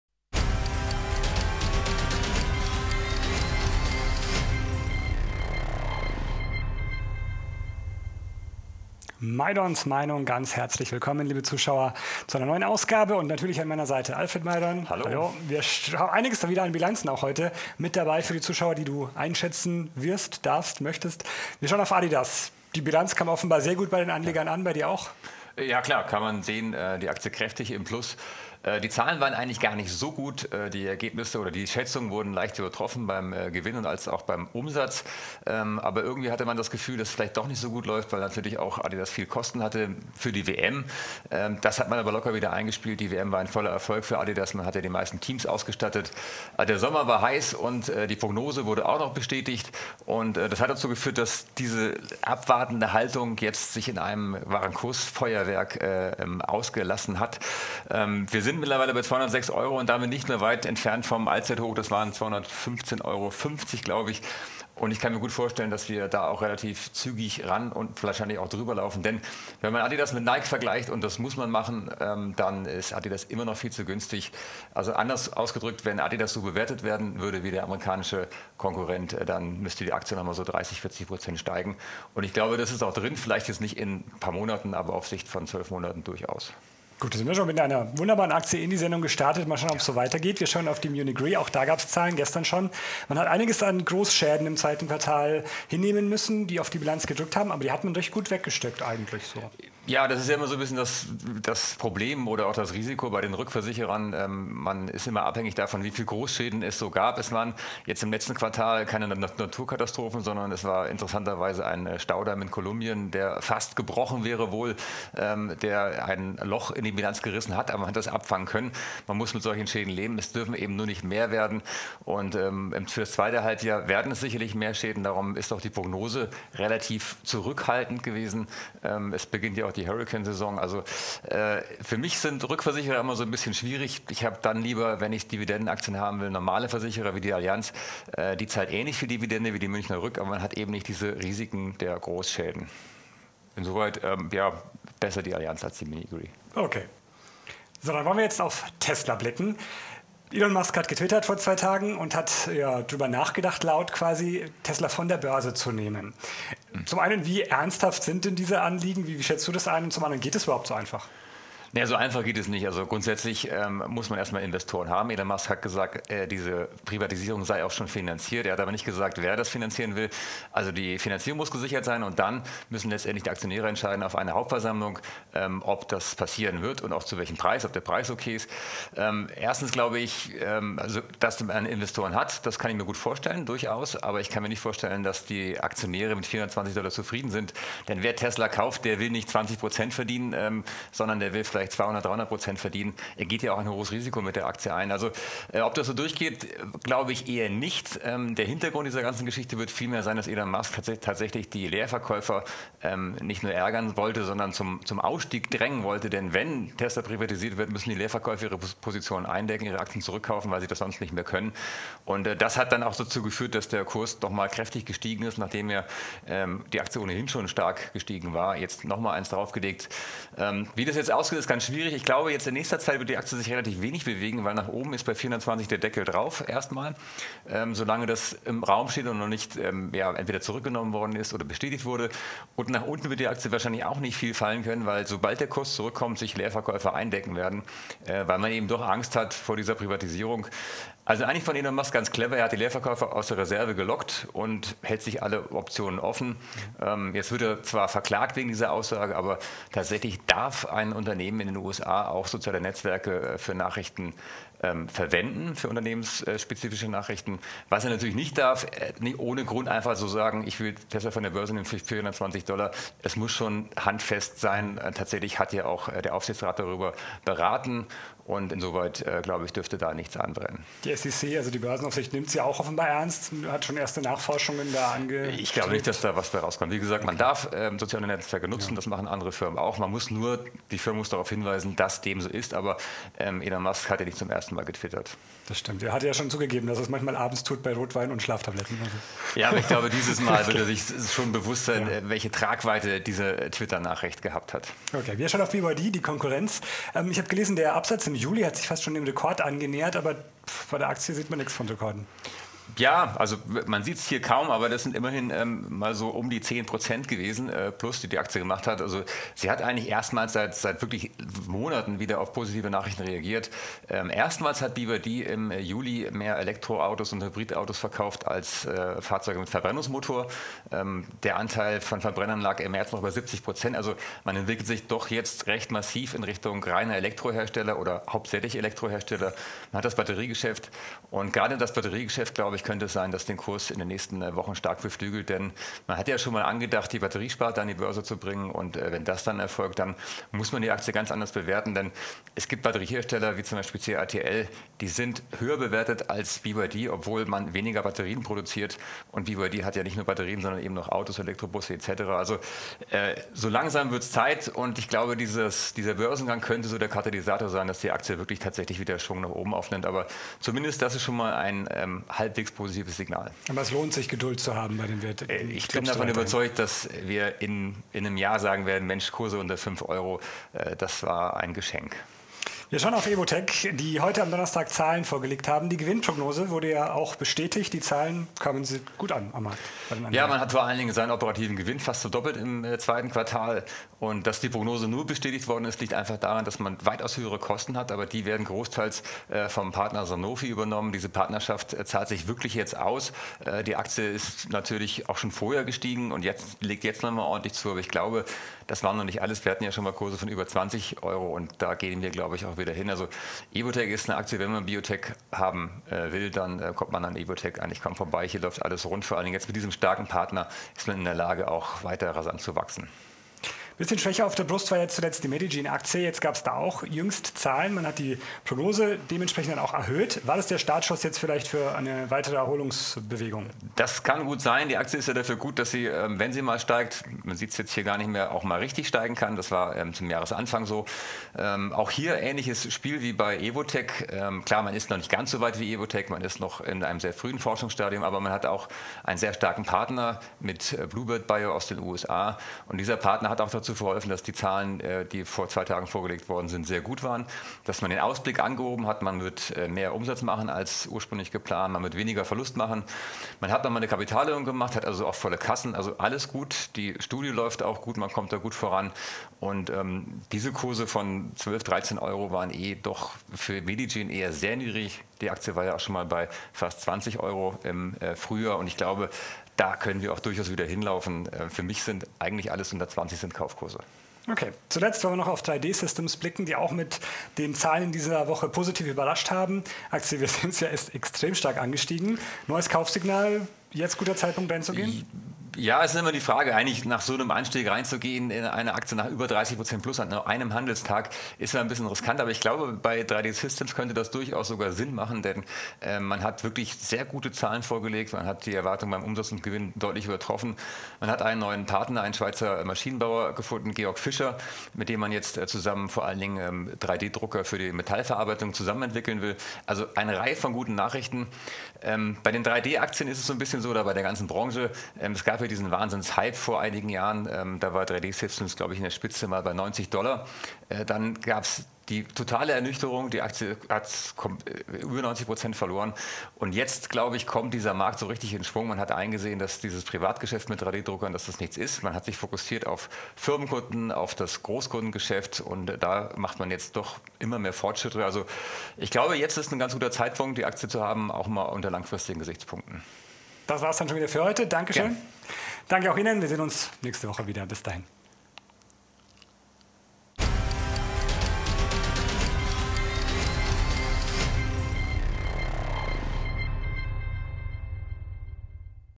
Maydorns Meinung: Adidas, Munich Re, Tesla, BYD, Evotec, Medigene, 3D Systems
In Maydorns Meinung analysiert Börsenexperte Alfred Maydorn die neuesten Entwicklungen der beliebtesten Aktien deutscher Anleger – und versieht sie mit seiner persönlichen Meinung.

In dieser Ausgabe geht es um Adidas, Munich Re, Tesla, BYD, Evotec, Medigene und 3D Systems.